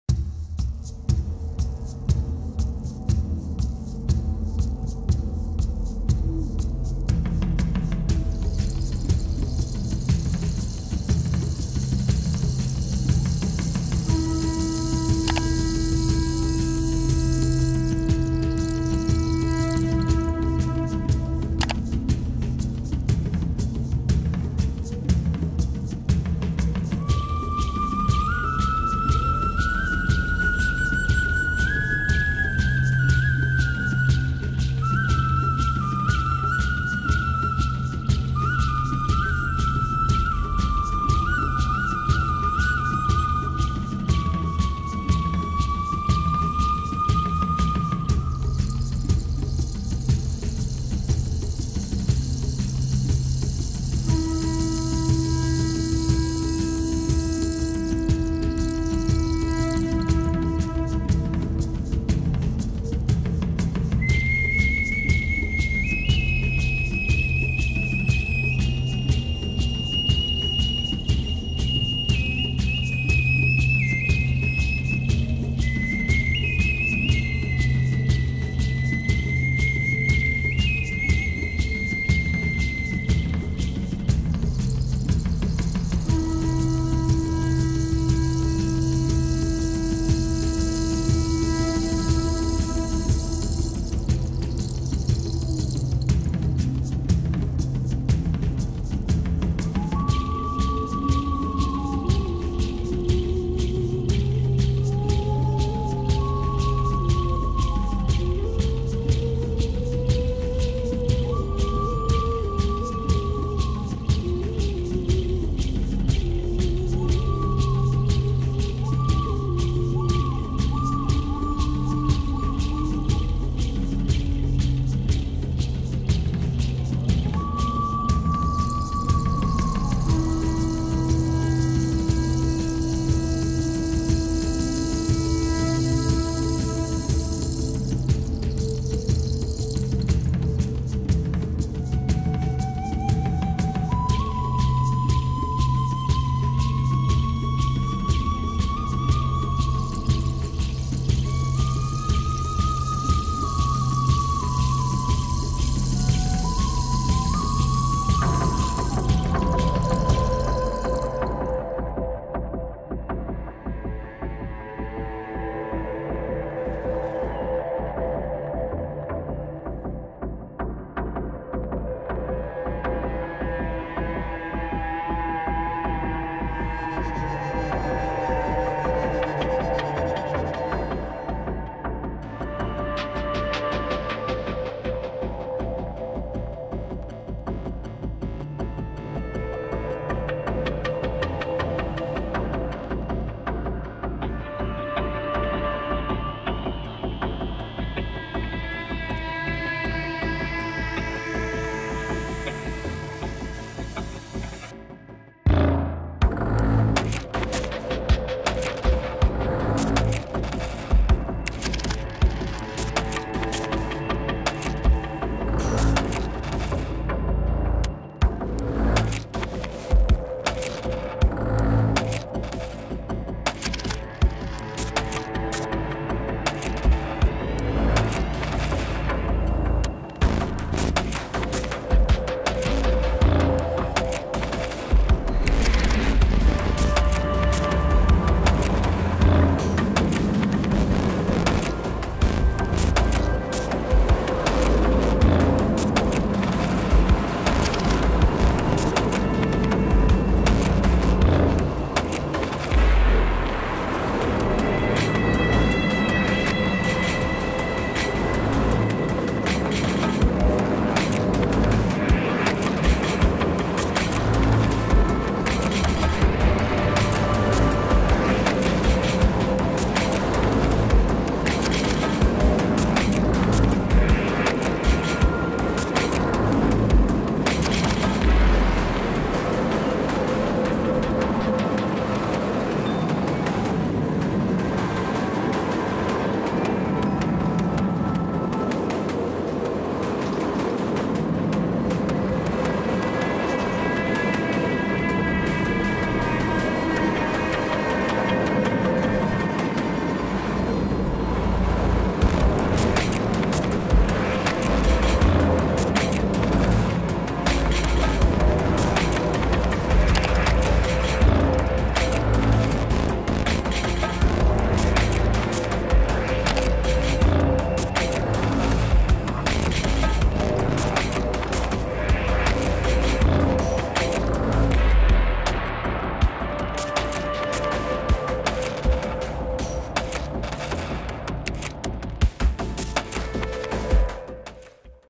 BRASIL GANHA NOVA MONTANHA RUSSA - SUCURI
Modelo de Coaster produzido para um concurso no canal @CorvusCoasters, buscando representar nosso país com um dos animais mais incríveis da nossa fauna: a Sucuri.